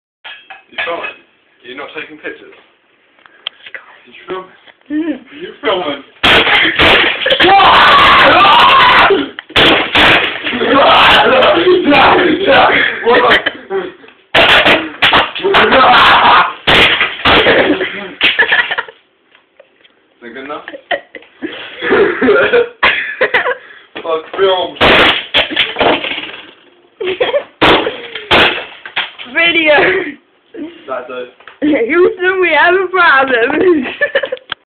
VCR SUCKS!
Mad me killing a VCR tape with a meat cleaver and a sausepan :)